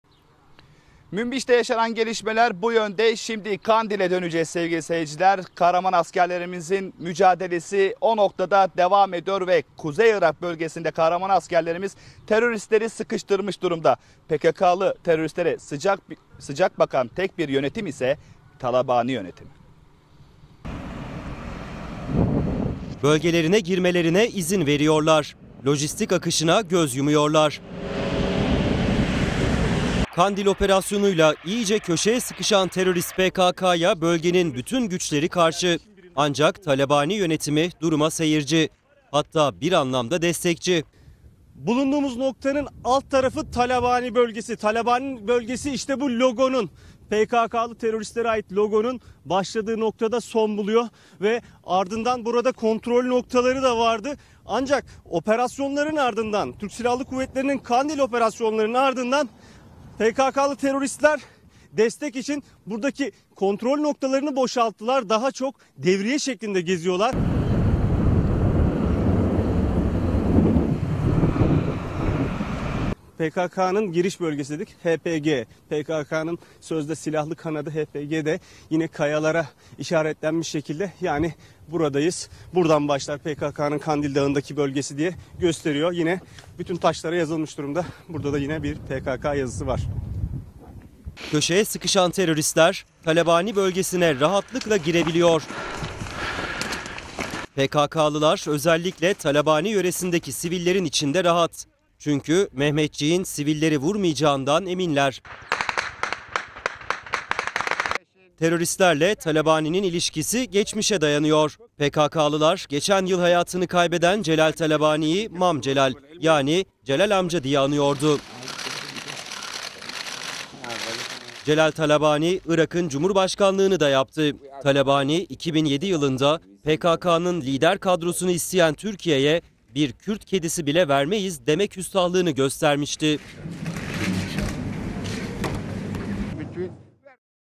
PKK'ya Talabani Desteği
Kuzey Irak'ta köşeye sıkışan terörist PKK'ya sıcak bakan tek bir güç var. O da Talabani Yönetimi.

Bölgelerine girmelerine izin veriyorlar. Lojistik akışına göz yumuyorlar.

TGRT HABER TV HAKKINDA:

● Türkiye'nin kanalıyız...